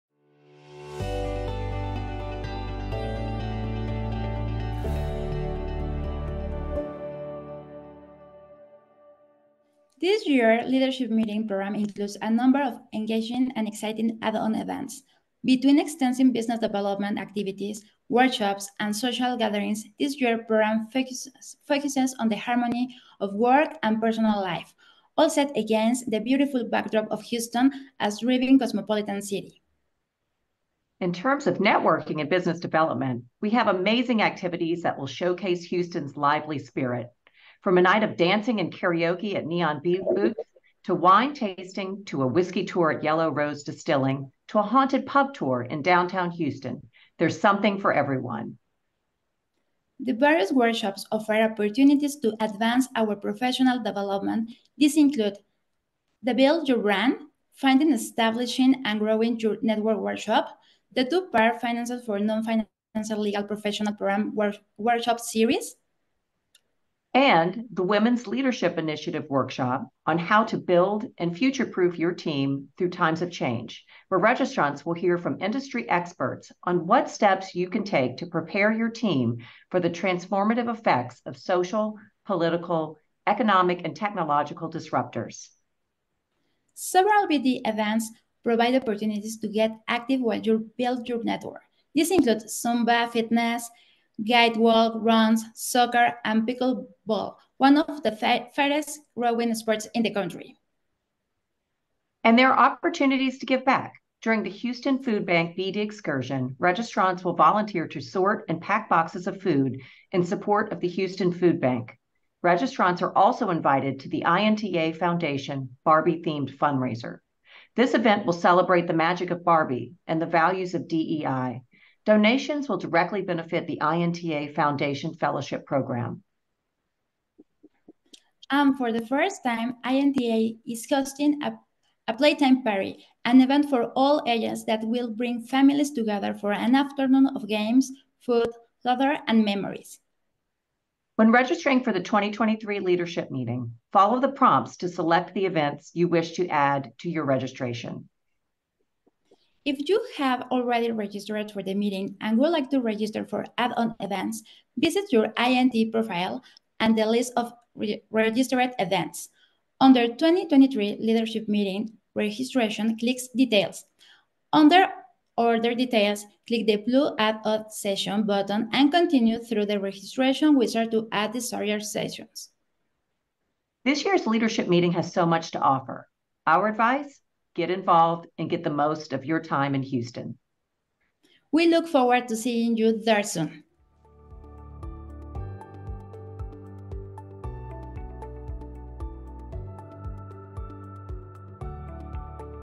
The 2023 Leadership Meeting Co-Chairs Discuss the Add-On Events and Business Development Activities
This year’s Leadership Meeting program emphasizes a holistic and balanced approach to the brand professional’s career progression.

Here, 2023 Leadership Meeting Co-Chairs Deborah Flores Garza (TelevisaUnivision, Mexico) and Melissa LaBauve (Munck Wilson Mandala, LLP, USA) discuss the Meeting's various Add-On events and Business Development activities.

Our annual Leadership Meeting is an invitation-only event for our Board and members who volunteer on committees and project teams. Join us to get involved with the Association,  participate in critical conversations, and help shape the future of the IP landscape.